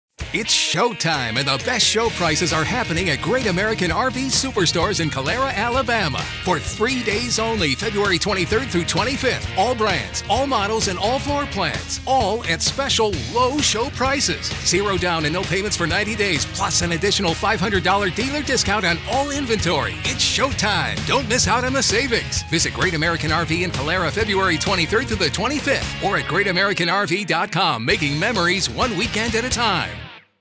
Exclusive Show Savings in Calera, AL - Happening This Weekend!
It's Show Time! Don't miss the fun happening this weekend at our Calera, AL dealership.

3 Days Only - February 23rd-25th
⭐ Exclusive Show Prices
⭐ Brand NEW 2024 Floorplans
⭐ Free Food, Free Parking

Hours:
Friday & Saturday - 8AM-6PM
Sunday - 10AM-5PM

Location:
1150 George Roy Parkway
Calera, AL 35040